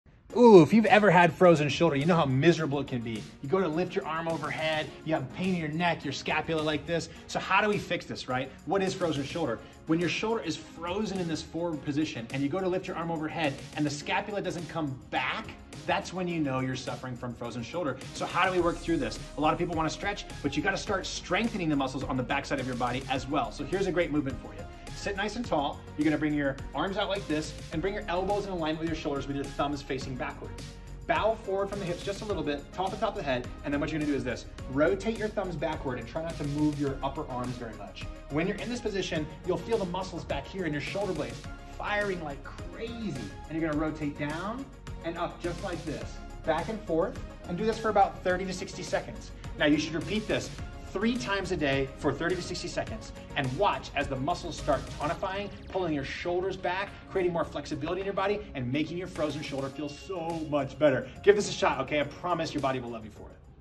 Fix Frozen Shoulder With This
If you experience frozen shoulder, try this to help stretch it out!